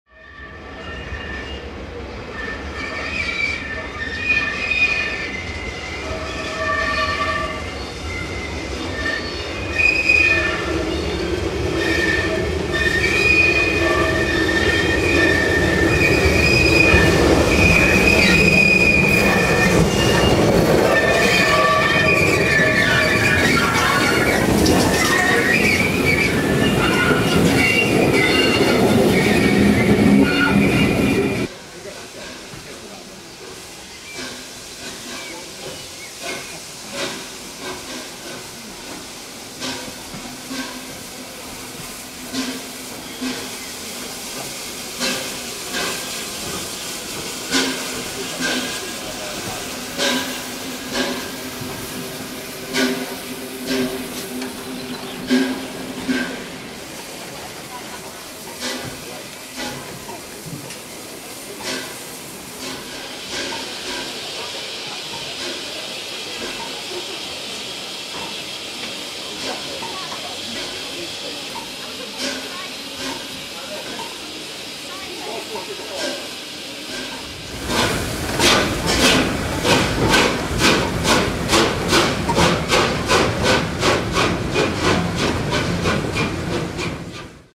A4 Bittern No 4464 arrives at the NYMR for the 2012 Spring Gala
Saturday 21st April 2012. A4 Class Bittern No 4464 arrives at the NYMR at 18:10 for the 2012 Spring Gala.